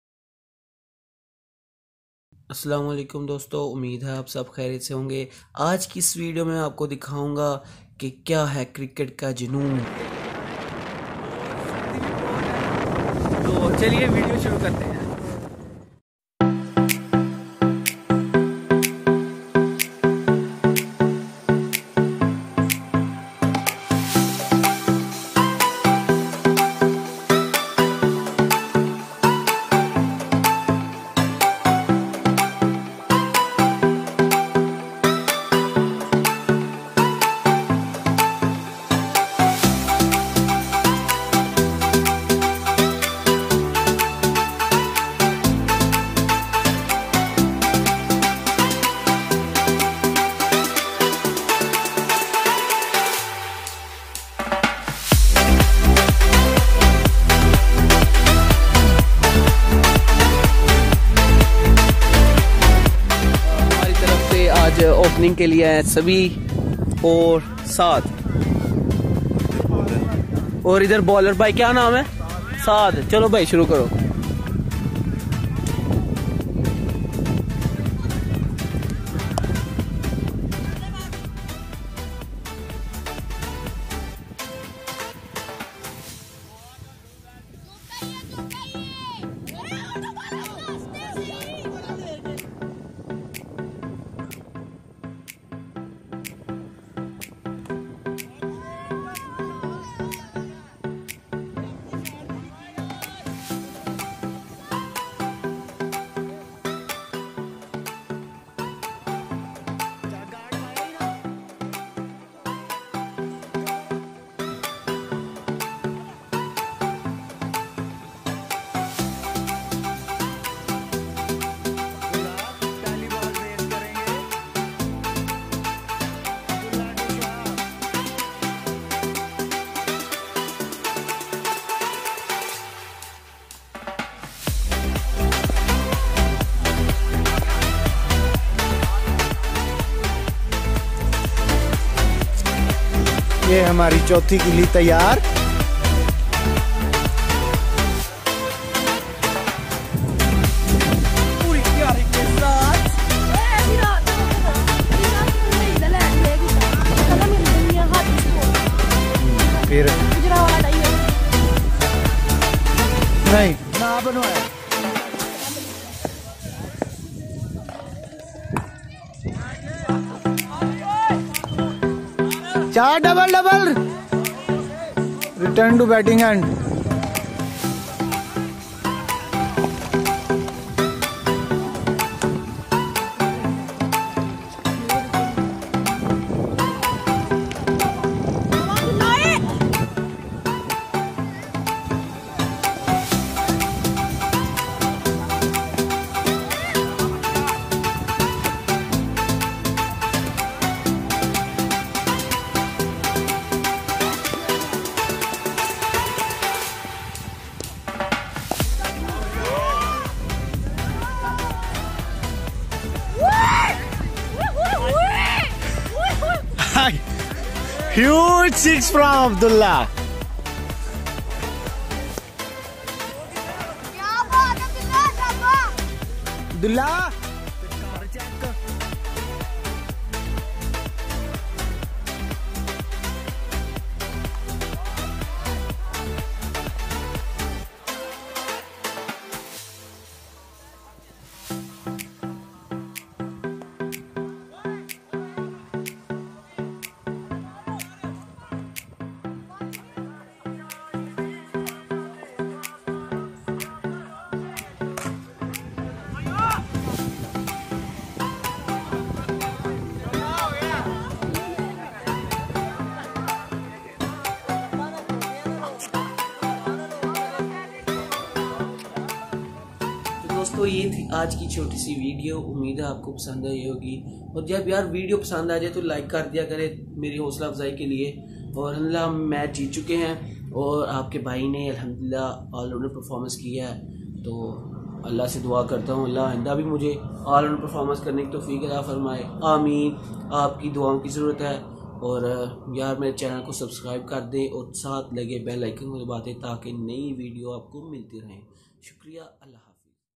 Vlog#2 Junoon e cricket
Ajj ki video my hum cricket khely gy